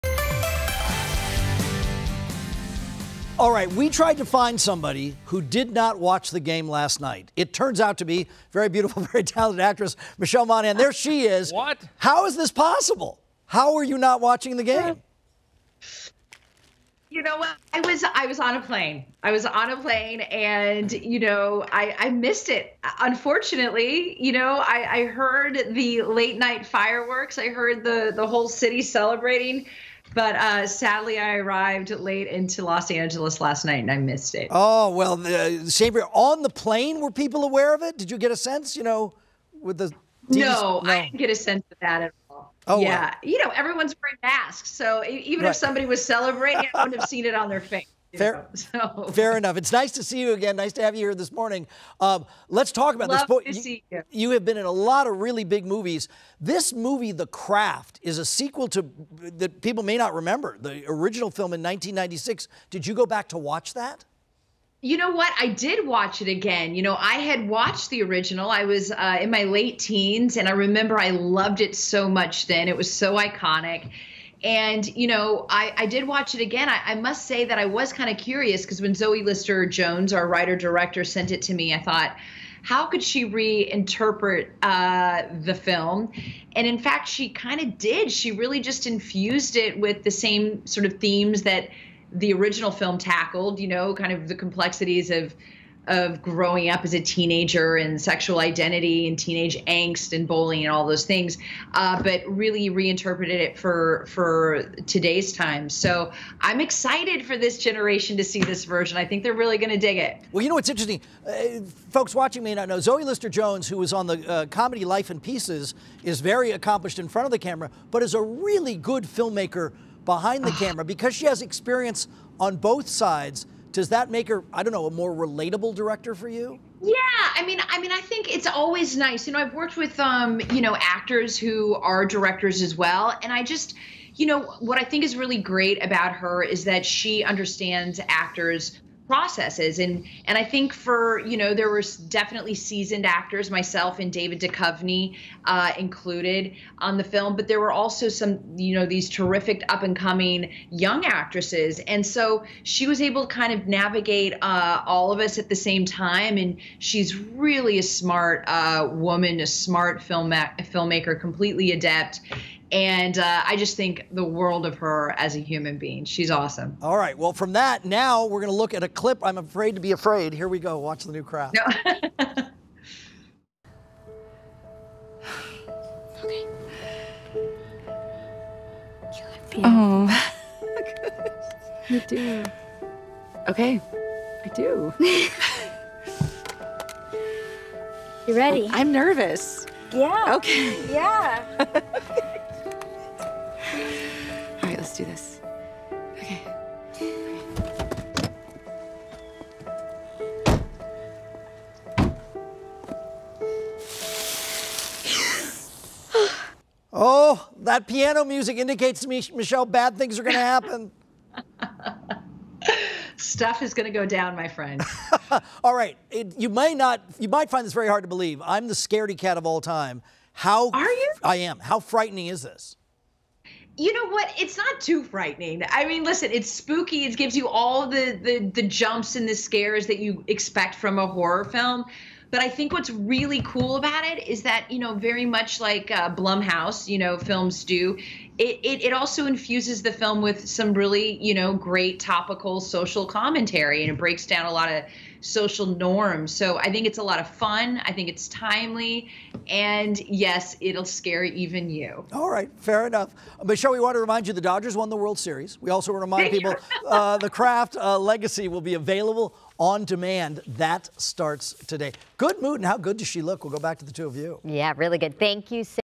Michelle Monaghan on the New Craft Movie "The Craft: Legacy"
"The Craft: Legacy" will be available On Demand starting today.
This segment aired on the KTLA 5 Morning News, Wednesday, October 28, 2020.